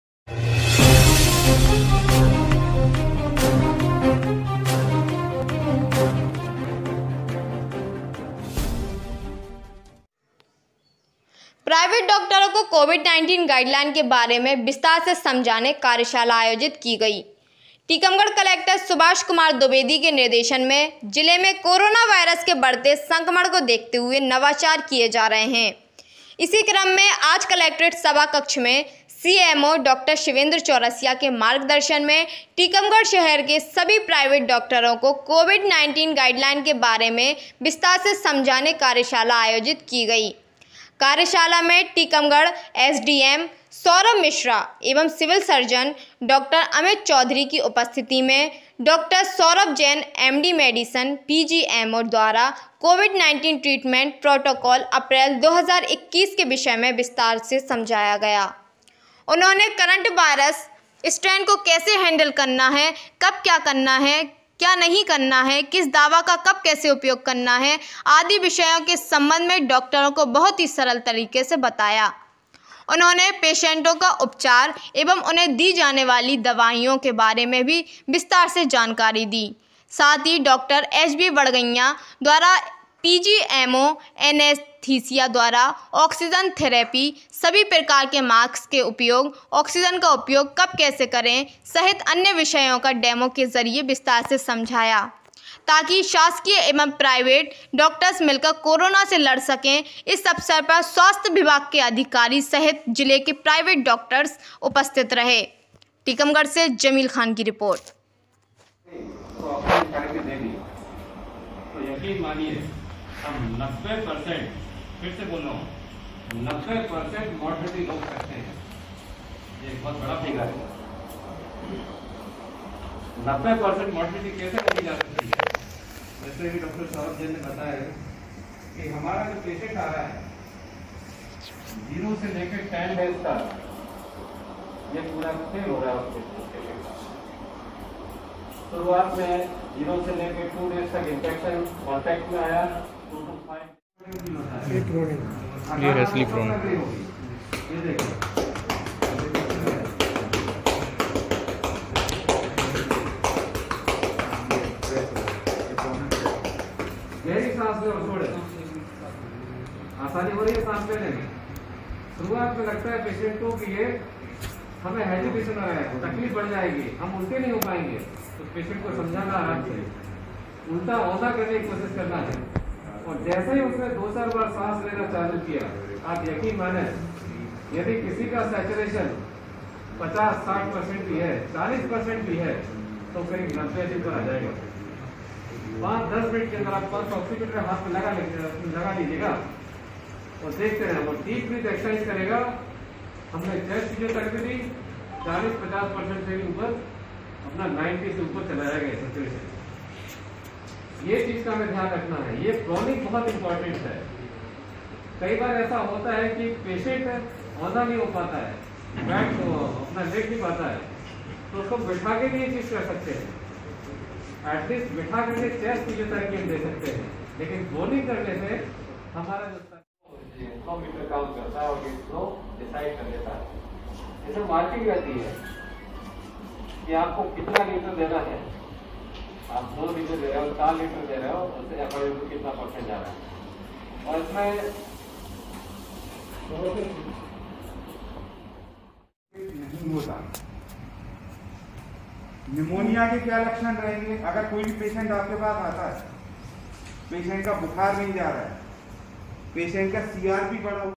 tikamgarh Covid-19 प्राइवेट डॉक्टरों को कोविड-19 गाईड लाईन को विस्तार से समझाने हेतु कार्यशाला
SRB Bulletin ! Madhya Pradesh ! datia news live! Gwalior news live ! up news live !

Live's :- Bhopal & Datia Madhya Pradesh India,

Home :- Datia, Distt- Datia, M.P

प्राइवेट डॉक्टरों को कोविड-19 गाईड लाईन के बारे में विस्तार से समझाने हेतु कार्यशाला आयोजित*
 *टीकमगढ़..कलेक्टर  सुभाष कुमार द्विवेदी के निर्देशानुसार जिले में कोरोना संक्रमण को दृष्टिगत रखते हुये नवाचार किये जा रहे हैं। इसीक्रम में आज कलेक्ट्रेट सभाकक्ष में सीएमओ डॉ. शिवेन्द्र चौरसिया के मार्गदर्शन में टीकमगढ़ शहर के सभी प्राइवेट डॉक्टरों को अपटेकिट कोविड-19 गाईड लाईन के बारे में विस्तार से समझाने हेतु कार्यशाला आयोजित की गयी।
कार्यशाला में एसडीएम टीकमगढ़  सौरभ मिश्रा एवं सिविल सर्जन डॉ अमित चौधरी की उपस्थित में डॉ. सौरभ जैन एमडी मेडिसन पीजीएमओ द्वारा कोविड-19 ट्रीटमेंट प्रोटोकॉल अप्रैल 2021 के विषय में विस्तार से समझाया गया। उन्होंने करन्ट वारयस स्ट्रेन को कैसे हैंडल करना है कब क्या करना है, क्या नहीं करना है, किस दवा का कब कैसे उपयोग करना है आदि विषयों के संबंध में बहुत ही सरल तरीके से बताया। उन्होंने पेशेंटों का उपचार एवं उन्हें दी जाने वाली दवाओं के बारे में विस्तार से जानकार दी।
 इसी के साथ ही डॉ. एचबी बड़गैंया द्वारा पीजीएमओ, एनएसथीसिया द्वारा ऑक्सीजन थैरेपी, सभी प्रकार के मास्क के उपयोग, ऑक्सीजन का उपयोग कब कैसे करें, सहित अन्य विषयों का डेमो के जरिये विस्तार से समझाया, ताकि शासकीय एवं प्राइवेट डॉक्टर्स मिलकर कोरोना से लड़ सके। इस अवसर पर स्वास्थ्य विभाग के अधिकारी सहित जिले के प्राईवेट डॉक्टर्स उपस्थित रहे।
टीकमगढ़ से जमील खान की रिपोर्ट